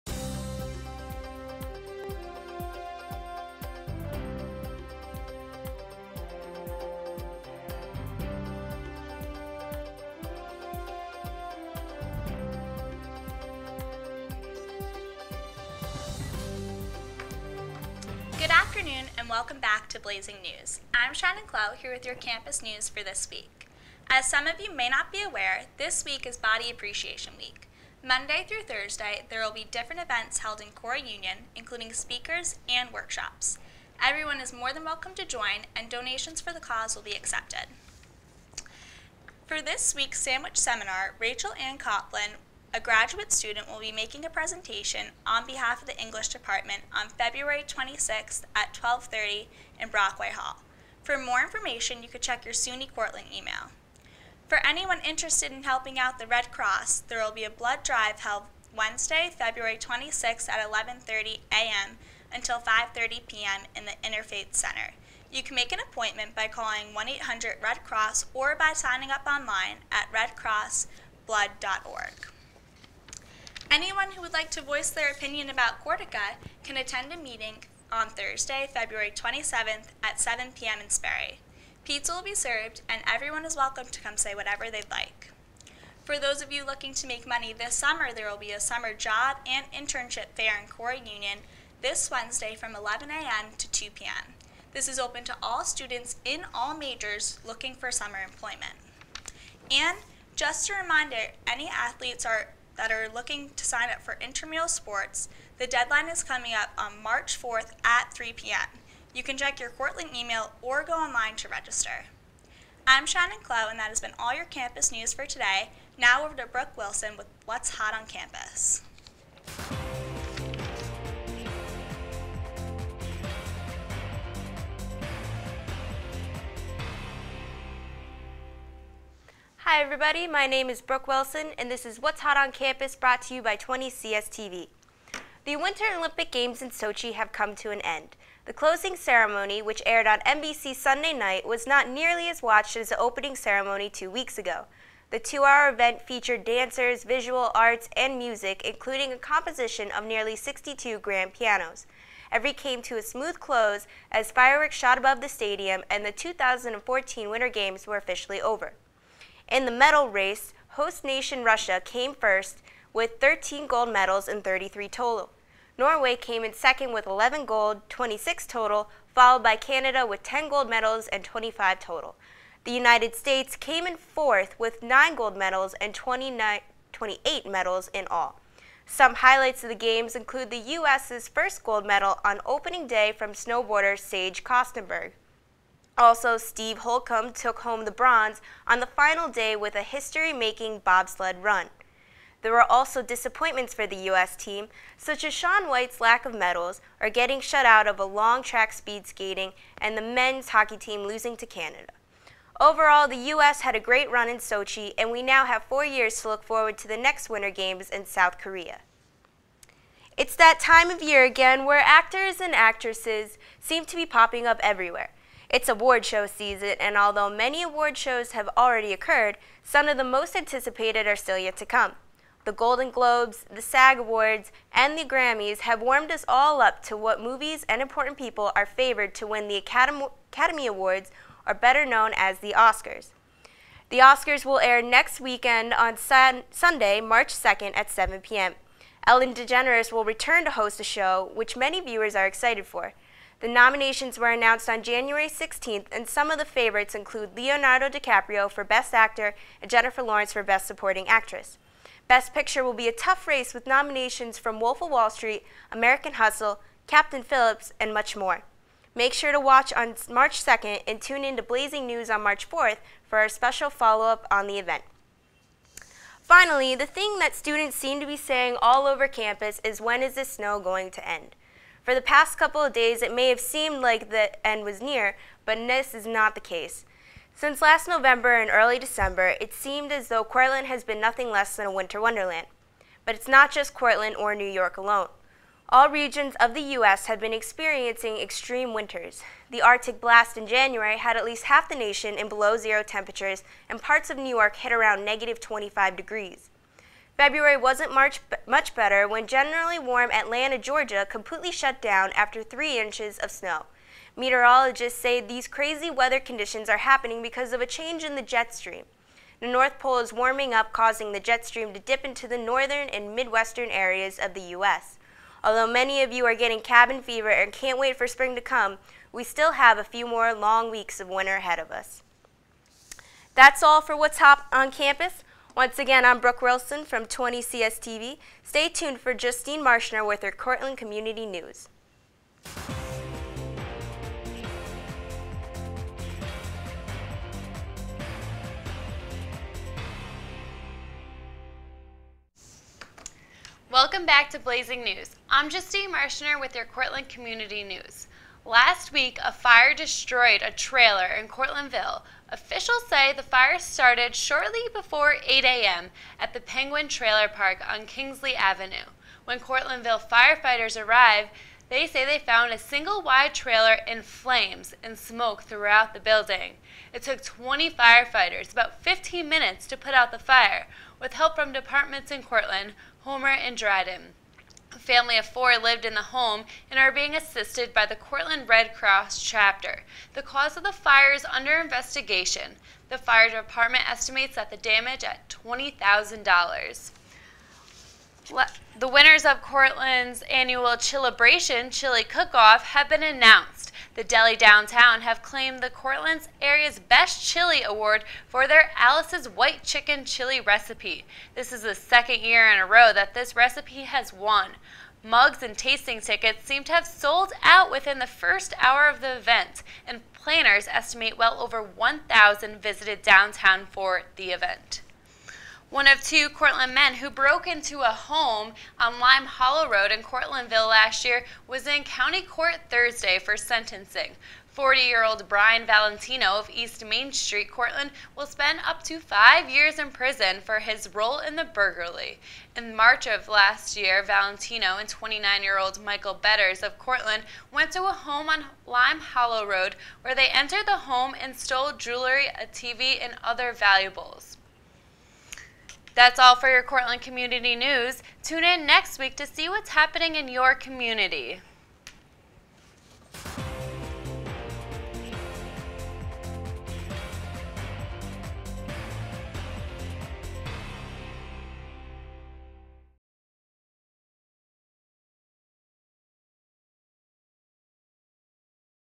Blazing News- 2/24/14
Blazing News is back this week with the hottest topics on campus! I hope you like the look of our anchors, seeing that we couldn't fit in the pictures this week! :)